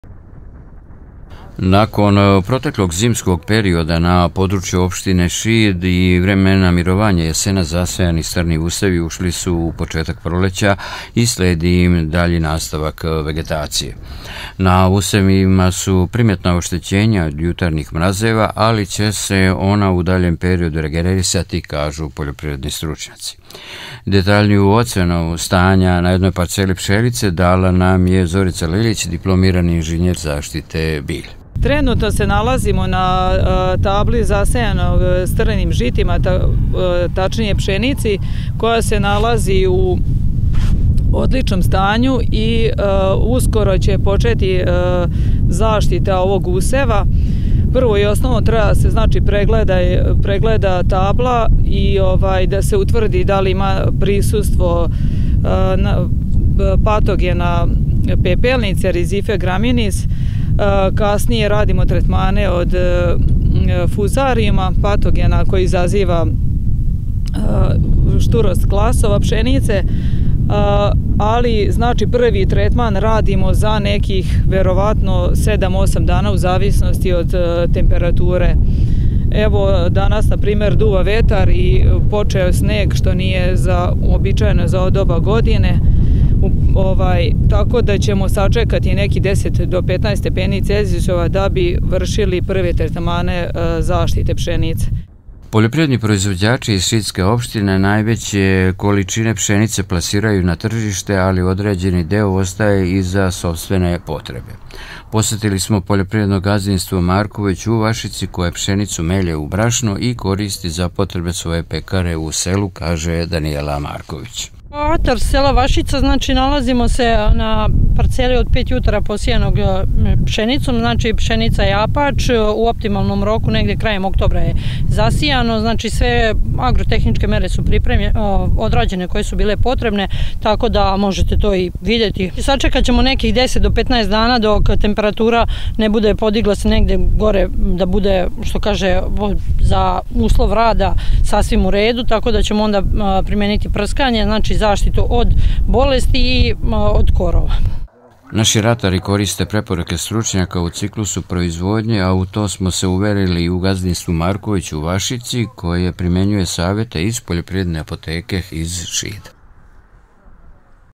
Stanje useva pšenice
Na području opštine Šid jesenas je zasejano 7000 hektara oranica u posedu poljoprivrednih gazdinstava i zemljoradničkih zadruga. Reč je o površinama pod strnim usevima i uljanom repicom koji su iz zimskog perioda izašli sa manjim oštećenjima od jutarnjih mrazeva. Kad je u pitanju dalja vegetacija, usevi će se regenerisati, ocena je poljoprivrednih stručnjaka.